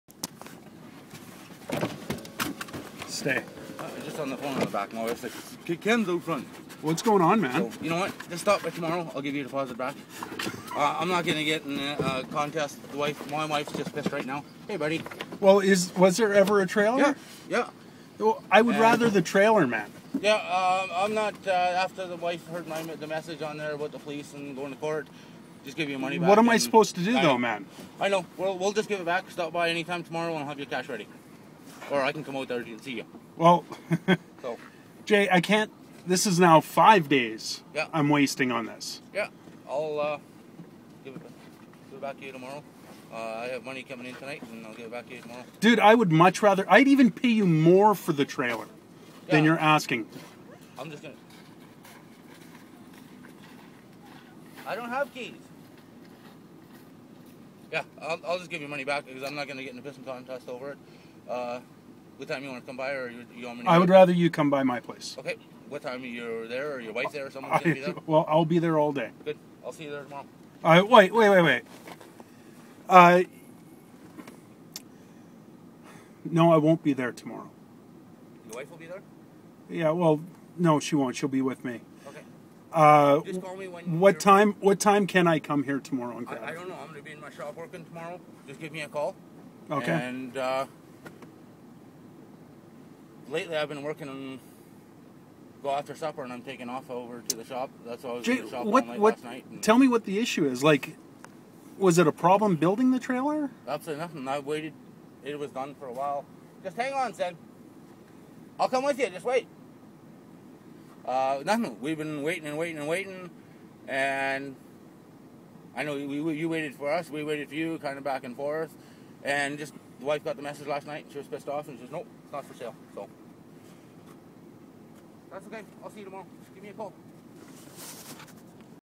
Jason McLellan Scam Trailers Abbotsford
Here is a video of dirt-bag Jason (John) McLellan who stole $1500 from me. He is telling me to "come by tomorrow to get my deposit"... Of course he hasn't given back and has since moved.

He claims he is a welder and will build trailers for you... He takes your money and pisses off.

The Abbotsford police will do NOTHING, even though there's verified 6 victims so far.

DO NOT, DO NOT DO BUSINESS WITH THIS FAT PIG!

I WILL GIVE $500 FOR ACCURATE INFORMATION ON WHERE THIS SCUMBAG NOW LIVES OR WORKS.